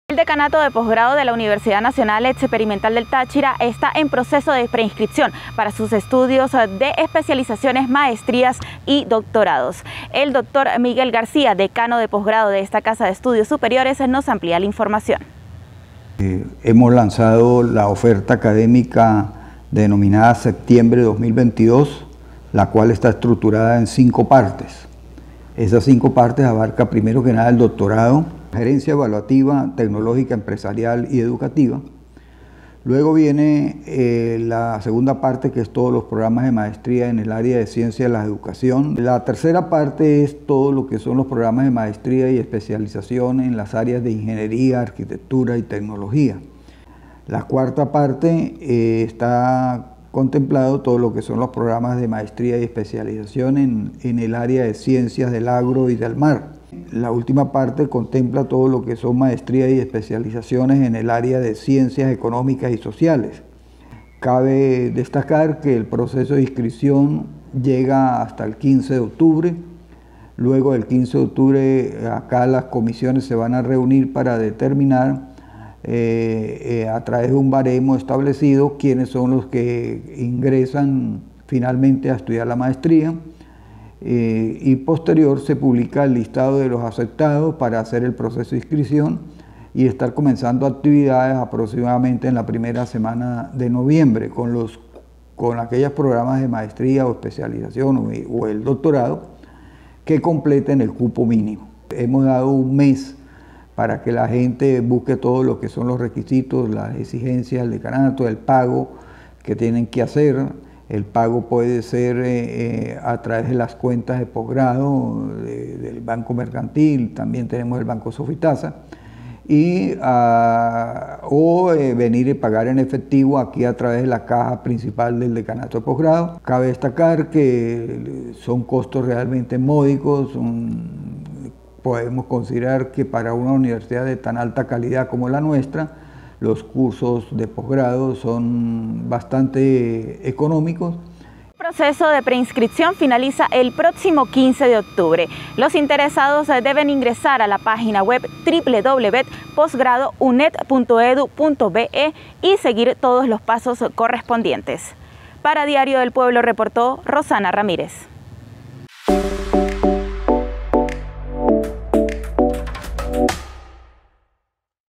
Doctorado, maestrías y especializaciones oferta el decanato de Postgrado UNET
El decanato de postgrado de la Universidad Nacional Experimental del Táchira está en proceso de preinscripción para sus diferentes programas en doctorados, maestrías y especializaciones.

Así lo informó el Dr. Miguel García decano de esta casa de estudios superiores quien explicó que está oferta académica 2022 está dividida en cinco partes.

La primera, con un doctorado en Gerencia Evaluativa, tecnología, empresarial y educativa.
La segunda, con maestrías en Ciencias de la Educación las cuales conforman maestría en: Docencia Virtual, Gerencia Educativa, Enseñanza y Aprendizaje de las Ciencias Básicas mención: Química y Biología así como también mención: Matemática y Física.

Así mismo la tercera parte comprende las maestrías en Informática, Electrónica, Industrial, Mantenimiento Industrial y Especialización en Gerencia de la Construcción. La cuarta corresponde a las Ciencias del Agro y del Mar, con las maestrías en Agronomía mención Producción Vegetal, Producción Animal, Especialización en Estudio y Evaluación del Impacto Ambiental.

La quinta y última parte comprende a las Ciencias Económicas y Sociales, allí ofertan las maestrías en: Administración de Instituciones de la Salud, Gerencia de Empresas Agricolas, Gerencia de Empresas mención Finanzas, Industria y Mercadeo, además de las especializaciones en Gerencia Pública y Orientación Familiar.

El proceso de preinscripción finaliza el próximo 15 de octubre, Los interesados en cursar alguna especialización, maestría o doctorado pueden acceder a la página web y seguir todos los pasos correspondientes.

Rosana Ramírez
C.N.P. 22.760